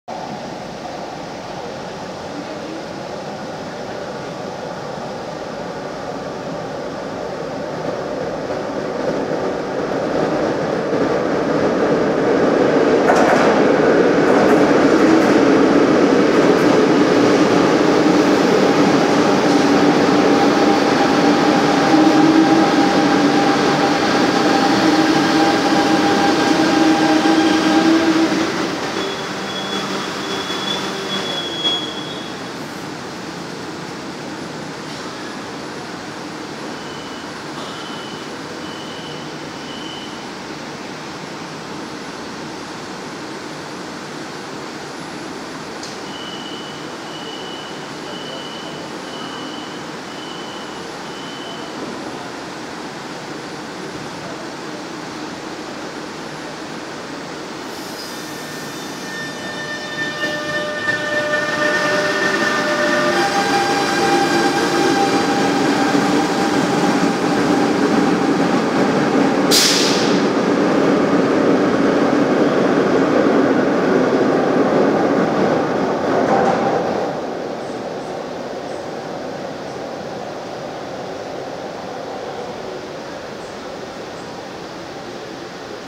Subte línea C - CNR Citic cabina 205 (Gral.San Martín)
Vemos llegar y partir rumbo a Constitución al CNR Citic cabina 205, el viernes 19 de julio de 2019 a las 17:58 hs.